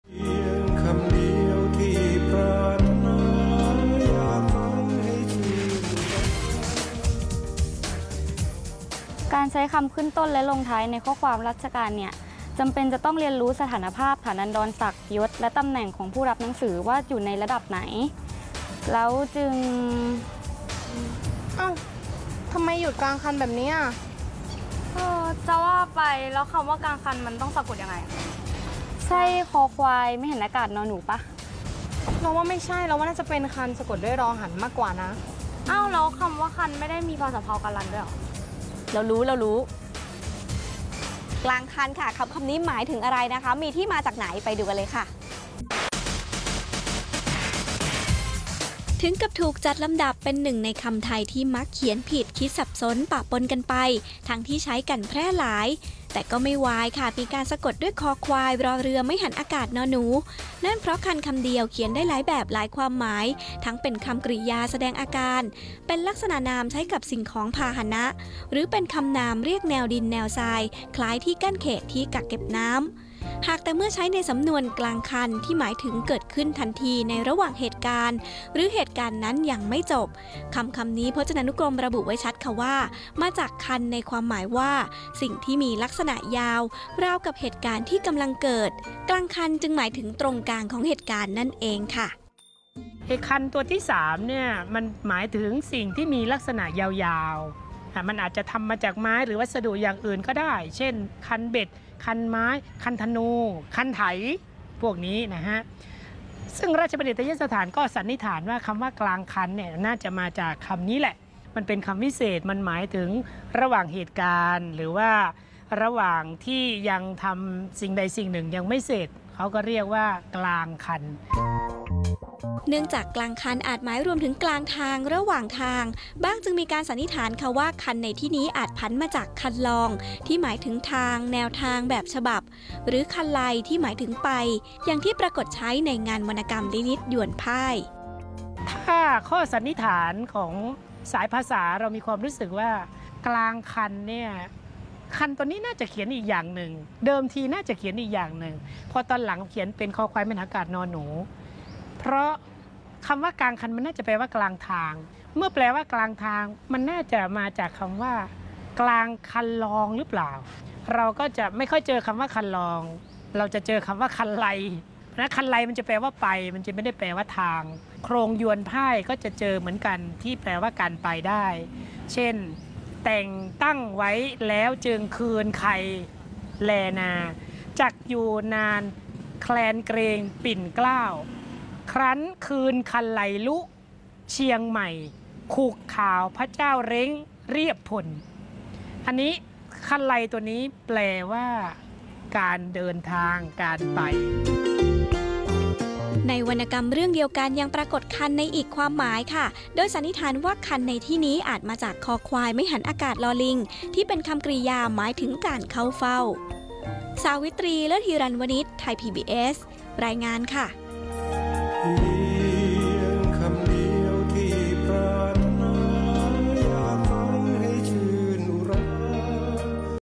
เพียงคำเดียว สะกดแบบไหนใน "กลางคัน"
รู้คำ รู้ความหมาย แต่เขียนไม่ได้ ส่วนหนึ่งเพราะคำๆเดียวอาจใช้ได้หลายแบบ หลายบริบท จึงมีไม่น้อยที่มักเขียนกันผิด หนึ่งในนั้น คือ "กลางคัน" คำวิเศษณ์บอกเหตุการณ์